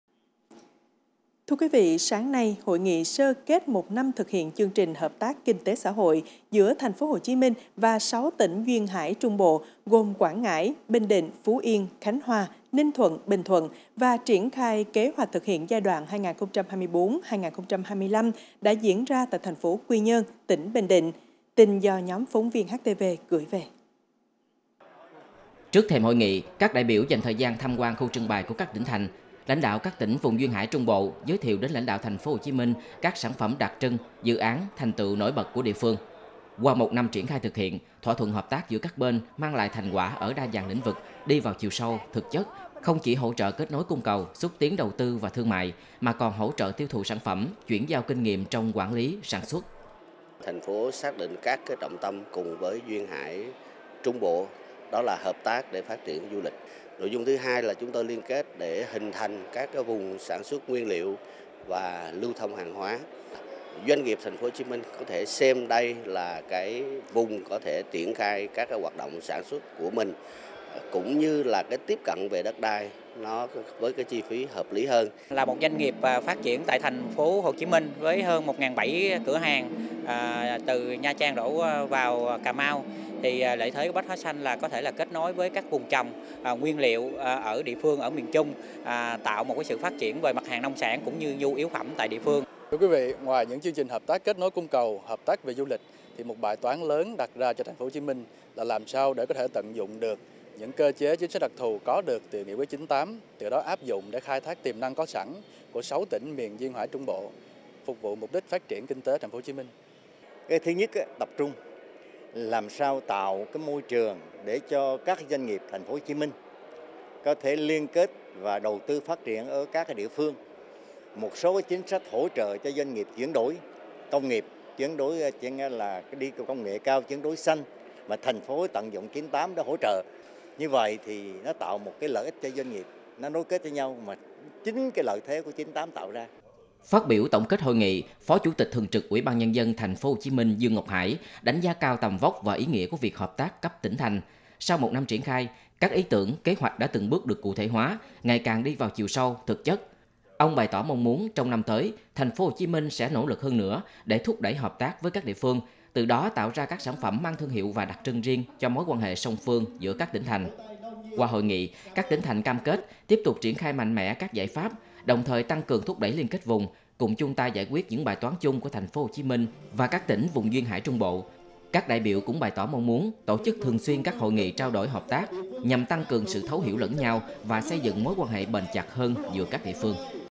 SƠ KẾT 1 NĂM HỢP TÁC PHÁT TRIỂN KINH TẾ - XÃ HỘI GIỮA TP.HCM VÀ 6 TỈNH DUYÊN HẢI TRUNG BỘ
Trước thềm hội nghị, các đại biểu dành thời gian tham quan khu trưng bày của các tỉnh thành. Lãnh đạo các tỉnh vùng Duyên hải Trung bộ giới thiệu đến với lãnh đạo TP.HCM các sản phẩm đặc trưng, dự án, thành tựu nổi bật của địa phương.